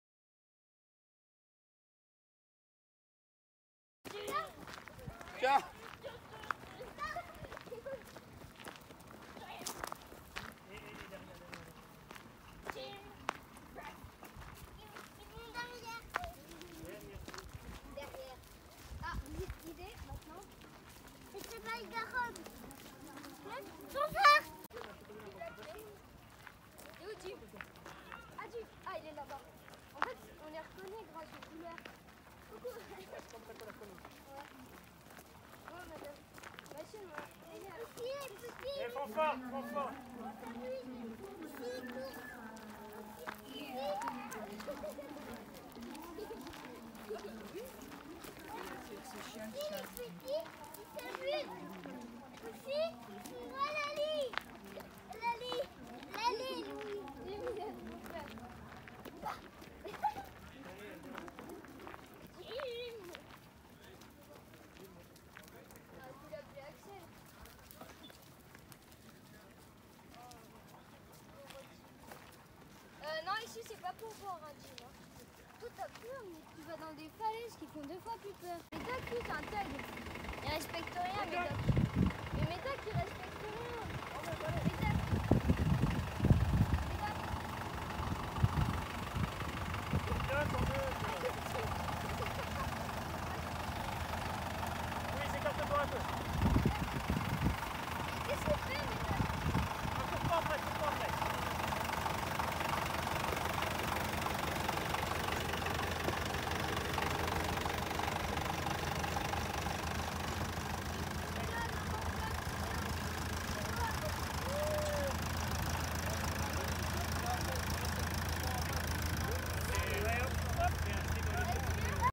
Fête de la ruralité St USUGE 2018
Présentation des chiens lors de la fête.